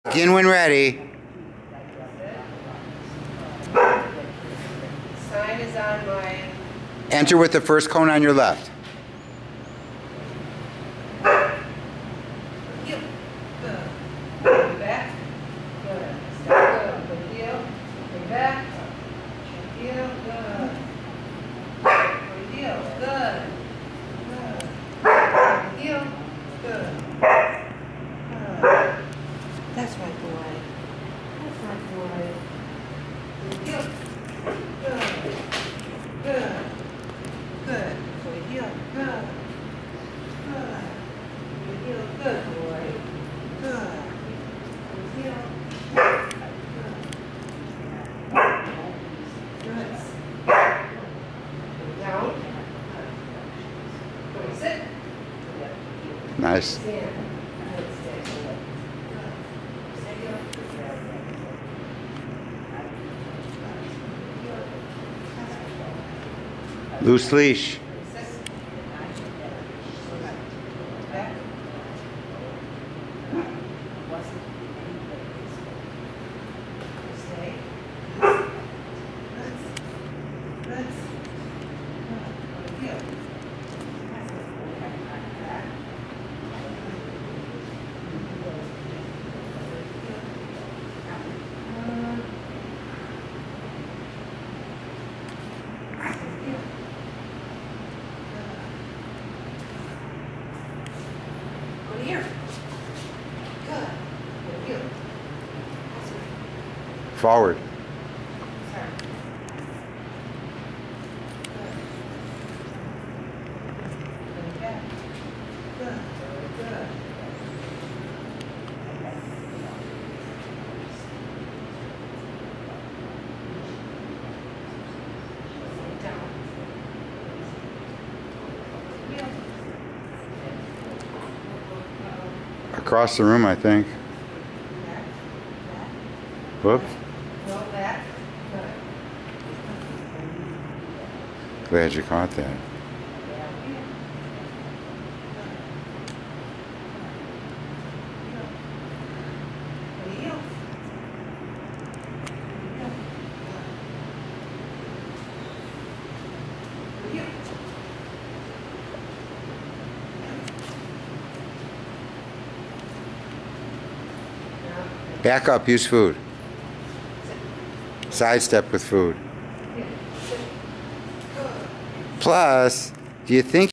Allan Ross Dog Training
Sherri and Cody practice a Cynosport Level 1 Rally Obedience course in the Canine College.  1